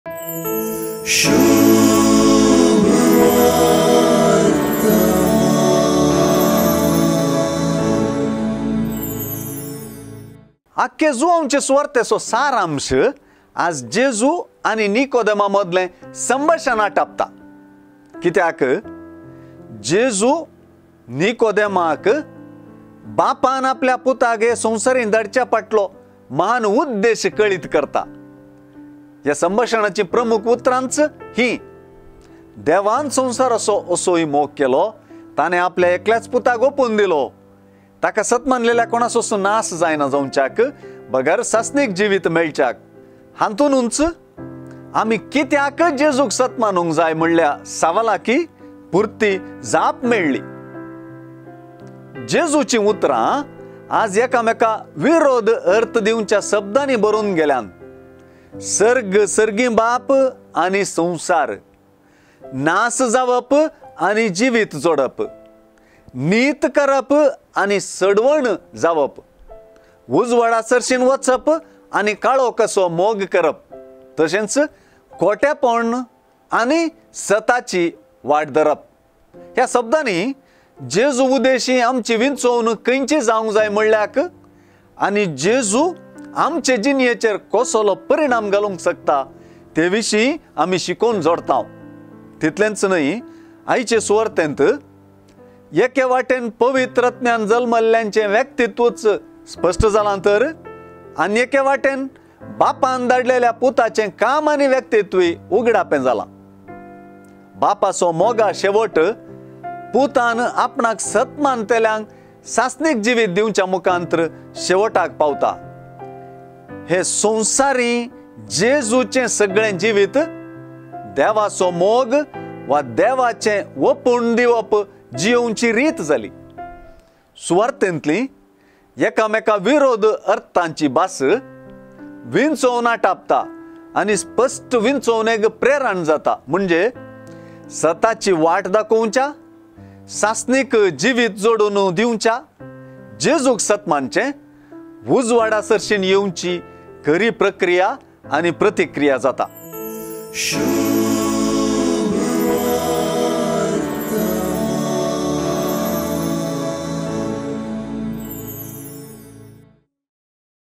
ಬಾಪಾನ್ ಪುತಾಕ್ ಕಿತ್ಯಾ ದಿಲೊ? | ಜುವಾಂವ್ 3:16-21 |ಶುಭ್‌ವರ್ತಮಾನ್ | 10 APR 24
SHUBHVORTOMAN: A TWO-MINUTE STIMULUS ON THE DAILY GOSPEL
ಶುಭ್‌ವರ್ತಮಾನ್: ದಿಸಾಚ್ಯೆ ಸುವಾರ್ತೆಚೆರ್ ದೋನ್ ಮಿನುಟಾಂಚೆಂ ಪ್ರೇರಣ್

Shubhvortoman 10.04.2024
ಪಾಸ್ಕಾಂಚೊ ದುಸ್ರೊ ಬುಧ್ವಾರ್
ಜುವಾಂವ್ 3:16-21
ಬಾಪಾನ್ ಪುತಾಕ್ ಕಿತ್ಯಾ ದಿಲೊ?
It is an initiative by Rev. Dr Vincent Sequeira,
Secretary, Commission for Bible
Diocese of Mangalore

Bishop of Mangalore

Rev. Fr Vijay Machado,
Director, Mangala Jyothi

Camera, Editing & Graphics
Rev. Fr Anil Ivan Fernandes
, Director, CCC

Tune & Music by
Rev. Fr Suraj J Lobo, Bejai

Produced at
Canara Communication Centre, Mangalore